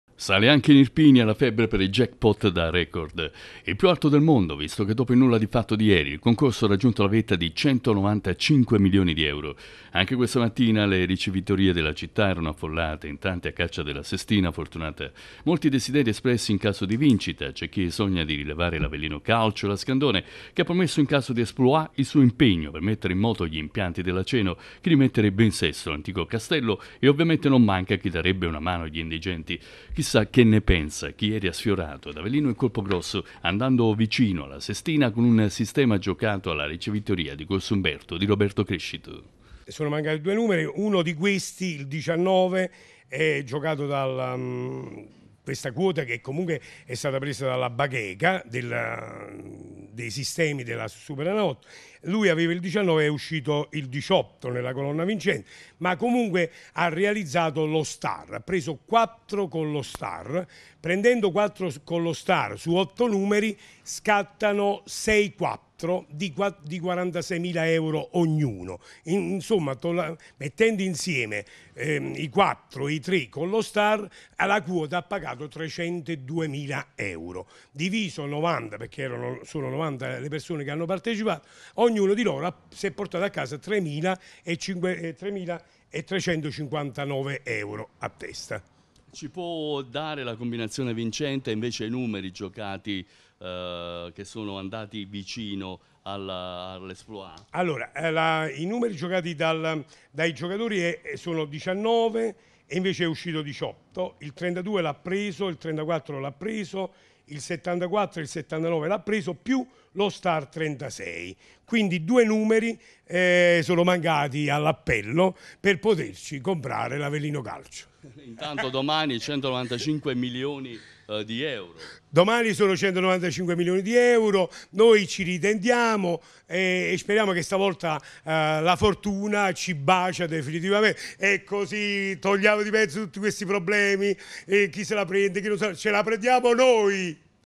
Anche ad Avellino è caccia alla sestina vincente per il jackpot da sogno
Anche in città sale la febbre per il jackpot più alto del mondo. Ricevitorie affollate anche questa mattina, vincita sfiorata presso il Sali e tabacchi di Roberto Crescito. Raggiunta la vetta dei 195milioni di euro